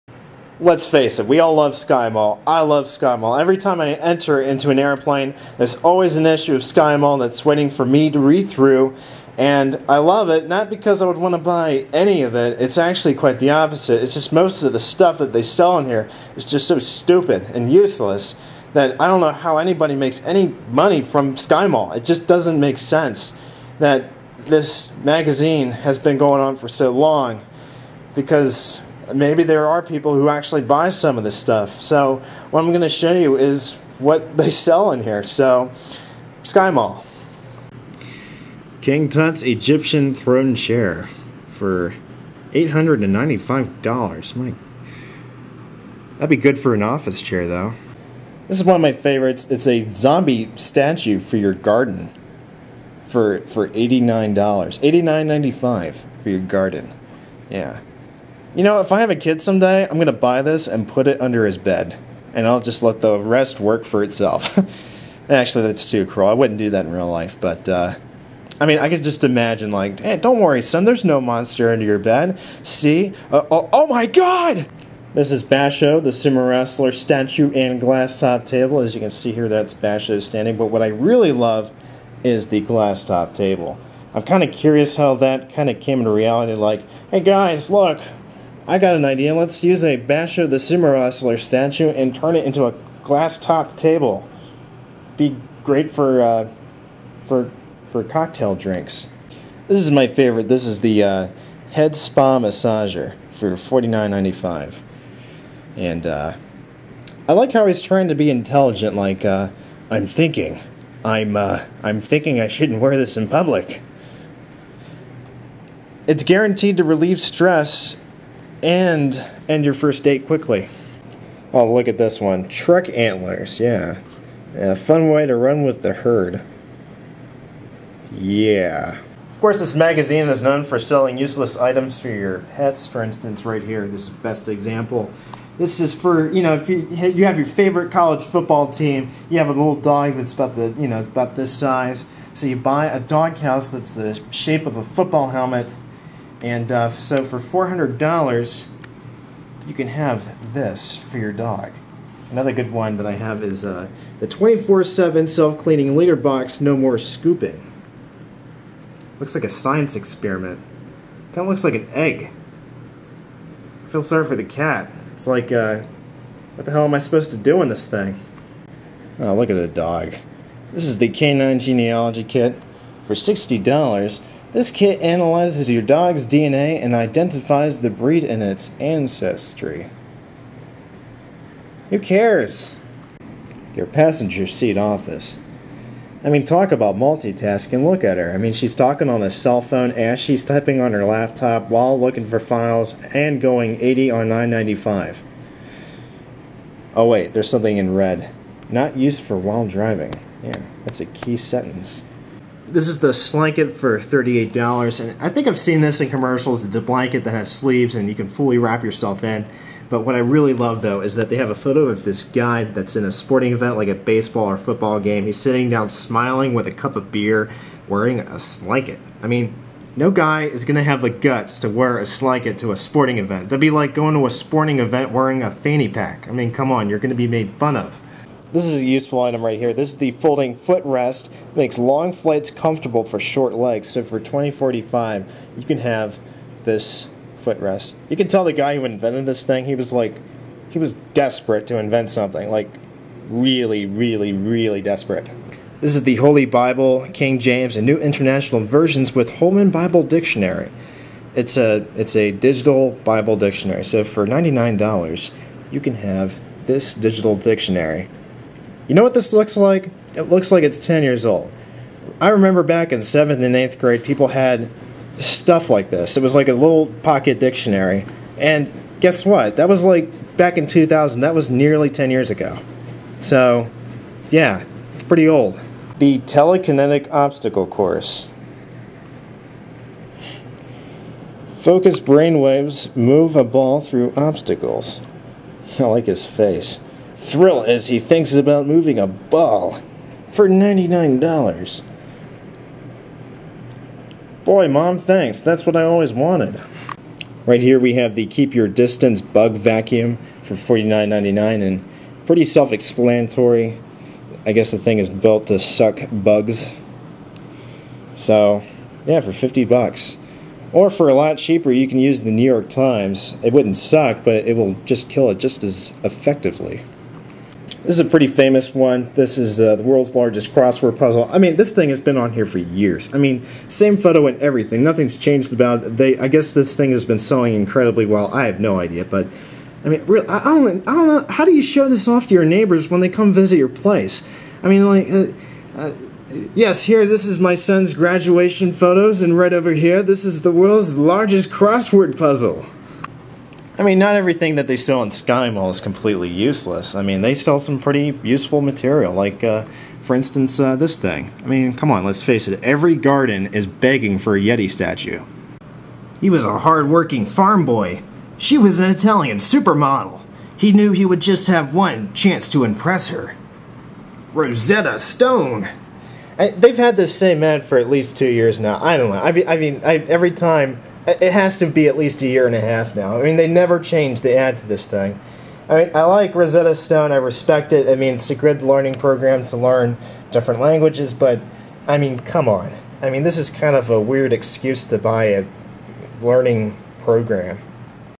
Sky Mall Magazine: Review
A magazine that sells some of the most useless crap, that it's actually fun to read.